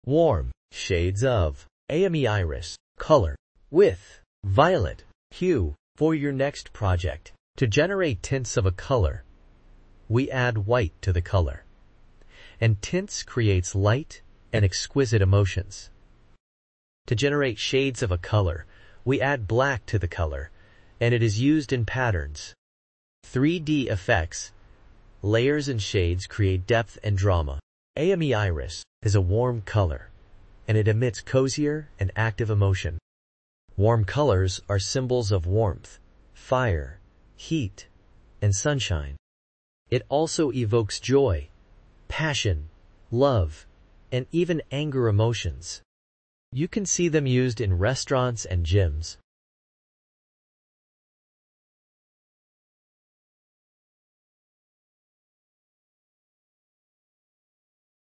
Shades & Tints of Ayame Iris color #763568 A Warm Violet color 682f5c 5b2950 854b78 #946189
Shades of Ayame Iris color #763568 Warm Shades of Ayame Iris color with Violet hue for your project!

Shades
Hex 682f5c

Tints
Hex 854b78

Ayame Iris is a warm color, and it emits cozier and active emotion. Warm colors are symbols of warmth, fire, heat, and sunshine. It also evokes joy, passion, love, and even anger emotions. You can see them used in Restaurants and Gyms.